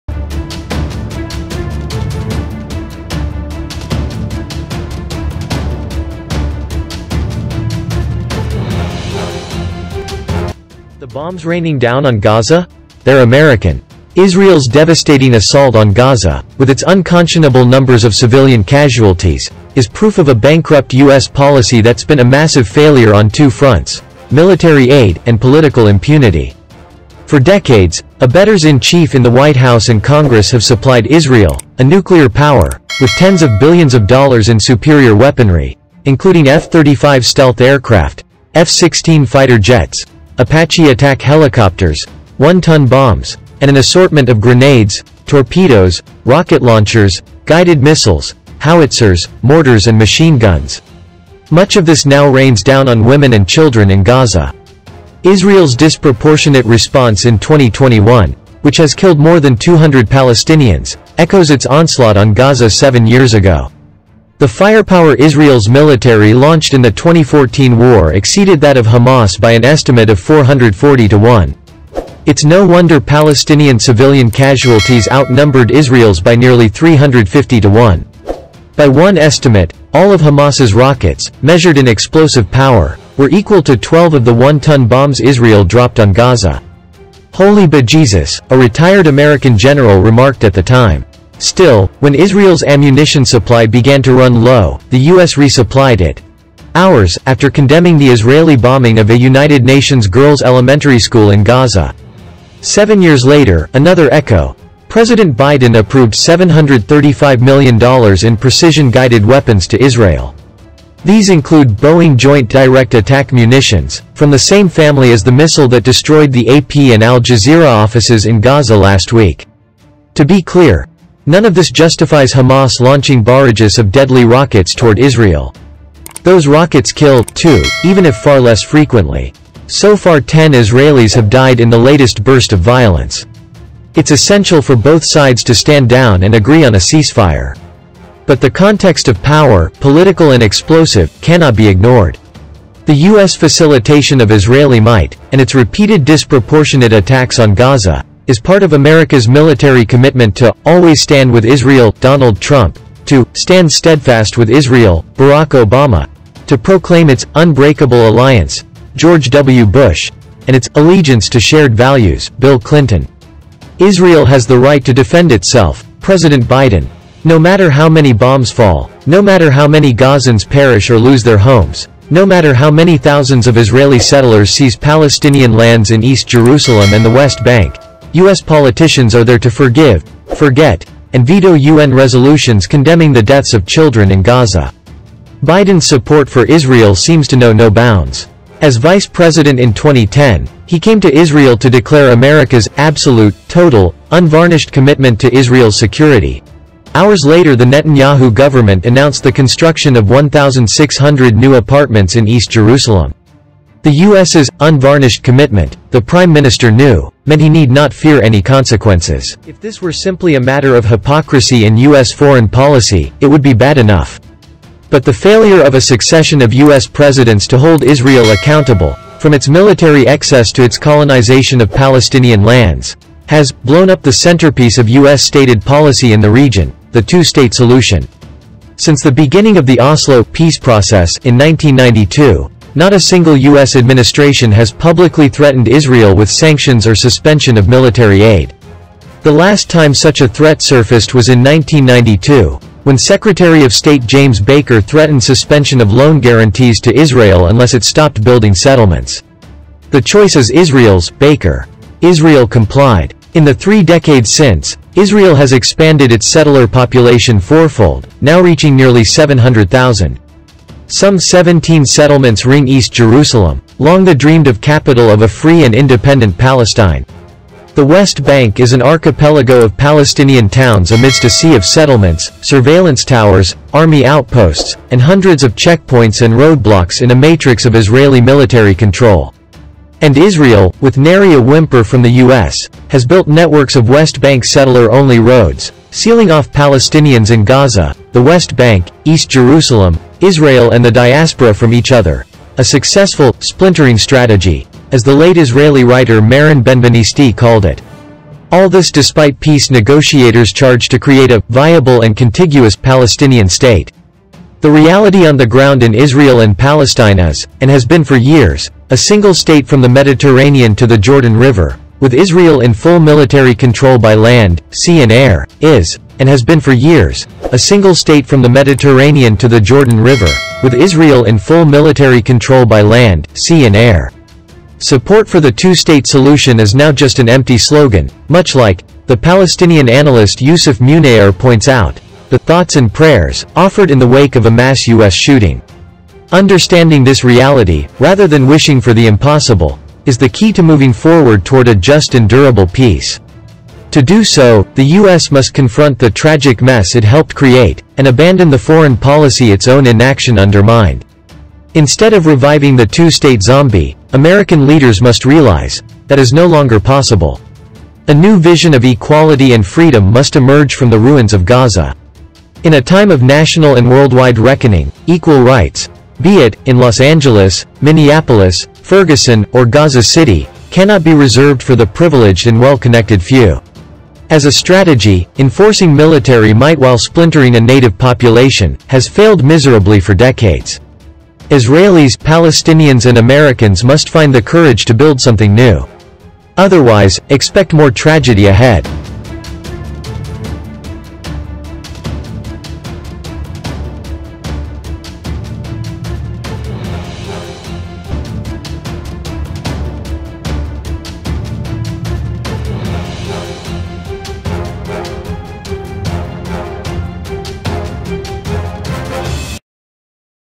Deadliest Israeli Bombing on Gaza || Are these American Bombs? - Reports
The Bombs Raining Down on Gaza?  They’re American

Israel’s devastating assault on Gaza, with its unconscionable numbers of civilian casualties, is proof of a bankrupt U.S. policy that’s been a massive failure on two fronts: military aid, and political impunity.

For decades, abettors-in-chief in the White House and Congress have supplied Israel, a nuclear power, with tens of billions of dollars in superior weaponry, including F-35 stealth aircraft, F-16 fighter jets, Apache attack helicopters, one-ton bombs, and an assortment of grenades, torpedoes, rocket launchers, guided missiles, howitzers, mortars and machine guns.

Much of this now rains down on women and children in Gaza.

Israel’s disproportionate response in 2021, which has killed more than 200 Palestinians, echoes its onslaught on Gaza seven years ago.

The firepower Israel’s military launched in the 2014 war exceeded that of Hamas by an estimate of 440-to-1.

It’s no wonder Palestinian civilian casualties outnumbered Israel’s by nearly 350-to-1.

By one estimate, all of Hamas’s rockets, measured in explosive power, were equal to 12 of the one-ton bombs Israel dropped on Gaza.

“Holy bejeezus,” a retired American general remarked at the time.

Still, when Israel’s ammunition supply began to run low, the U.S. resupplied it—hours after condemning the Israeli bombing of a United Nations girls’ elementary school in Gaza.

Seven years later, another echo: President Biden approved $735 million in precision-guided weapons to Israel.

We share Defence News Daily. Thanks,